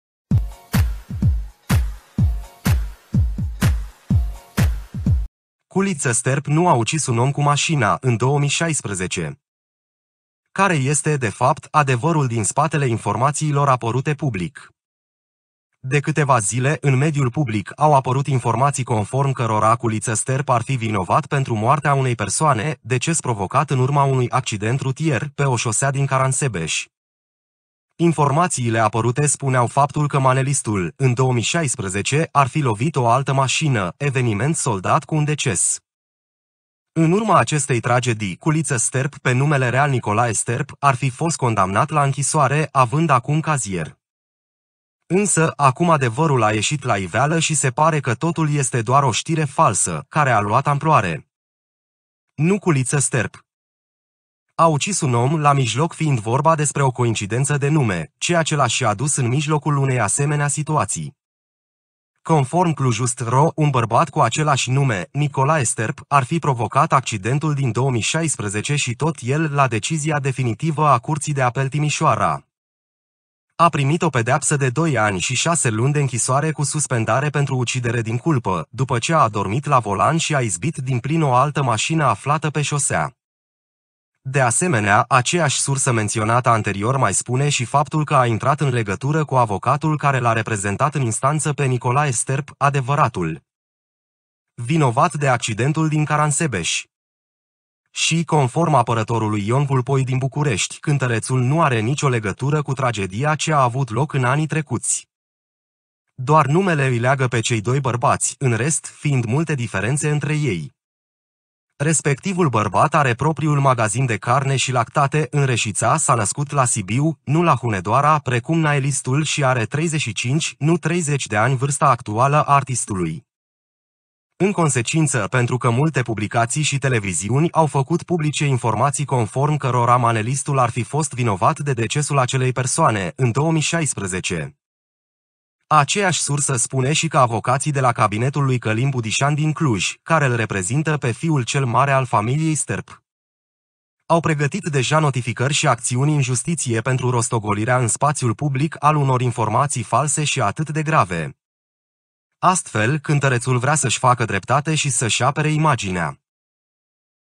Culiță Sterp nu a ucis un om cu mașina. Care este, de fapt, adevărul
De câteva zile, în mediul public au apărut informații conform cărora Culiță Sterp ar fi vinovat pentru moartea unei persoane, deces provocat în urma unui accident rutier, pe o șosea din Caransebeş.